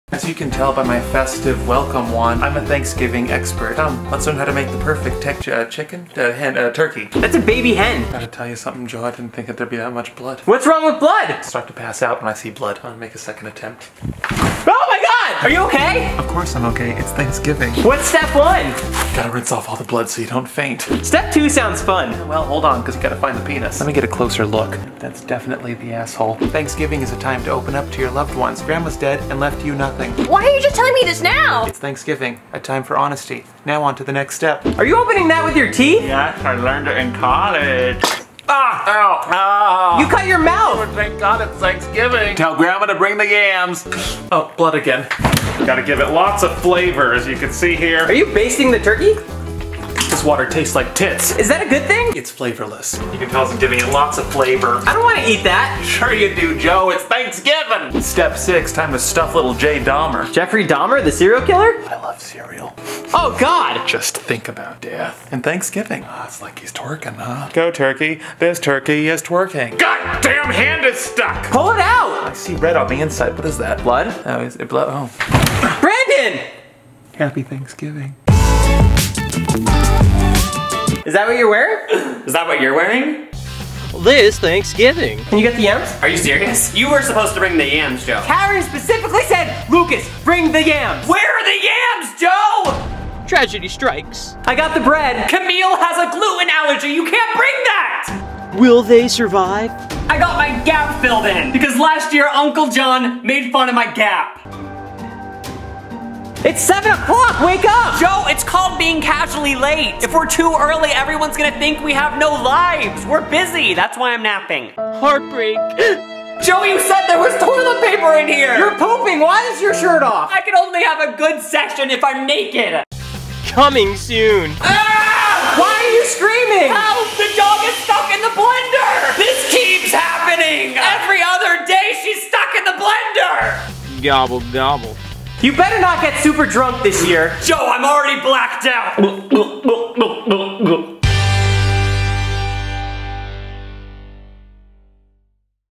BRANDON ROGERS DOES TURKEY DAY!!
2 Sketches, 1 Joe.

1st - Brandon does turkey day. He shows us how to cook a giant turkey and it is GLORIOUS! Seriously this turkey is like 40 pounds! It's CAAAARAZY

2nd Sketch - Lucas and I put together one of the greatest thanksgiving trailers OF ALL TIME! Its Gobbletacular

FOLLOW MY FRIENDS ON INSTAGRAM FROM THESE VIDEOS!!

Remember, just cause some turkey fell on the floor, doesn't mean